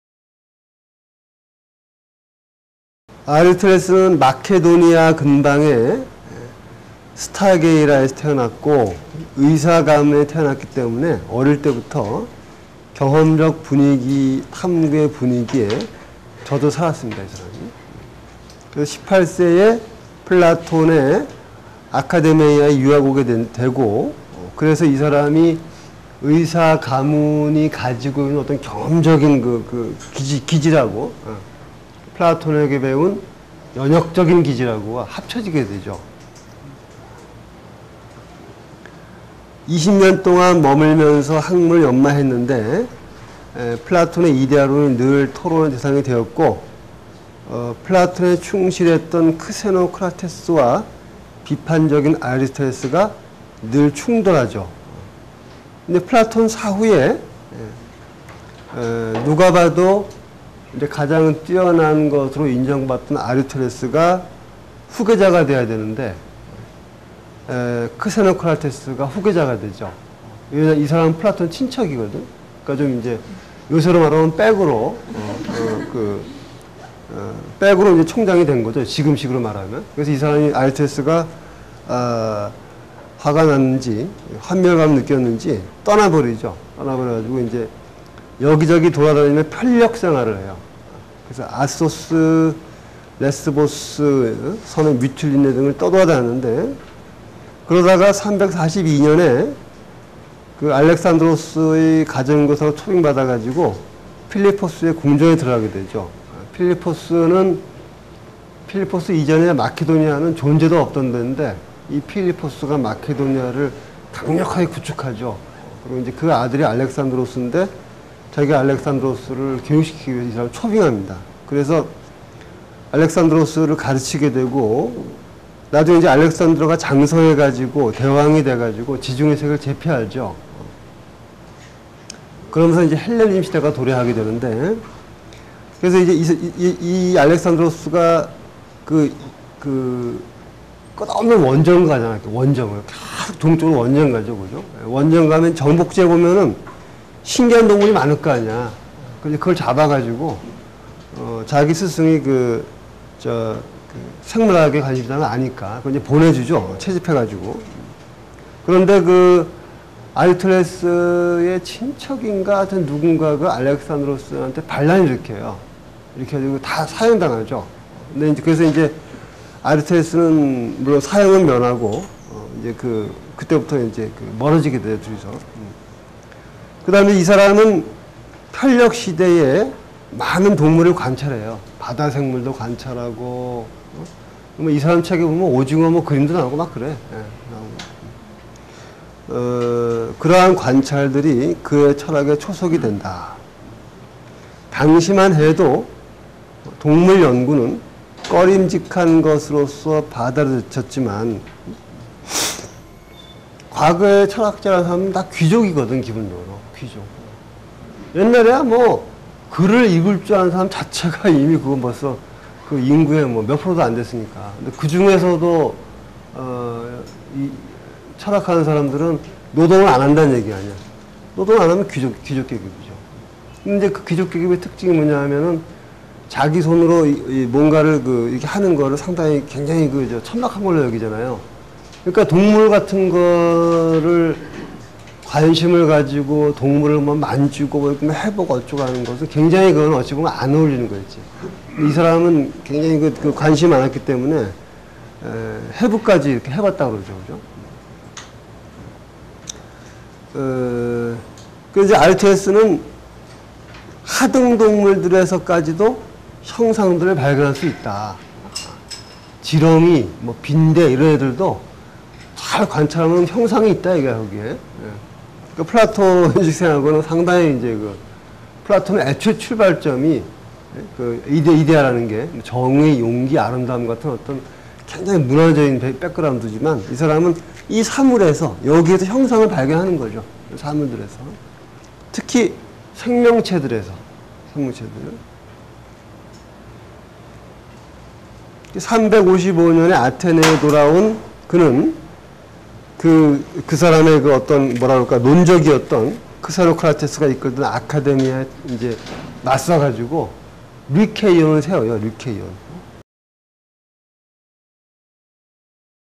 알렉산더 대왕의 가정교사 아리스토텔레스? - 이정우(철학자, 경희사이버대 교수)
플라톤의 제자이자 알렉산더 대왕의 스승이었던 고대 그리스의 철학자로, 철학을 비롯한 과학·미학·논리학·정치학 등 다방면에 걸쳐 방대한 양의 저술과 연구를 남긴 아리스토텔레스(Aristoteles, B.C 384~322)의 삶을 쫓아가 본다.

관련 강좌: 철학사 입문코스 Ⅱ _ 최초의 철학체계들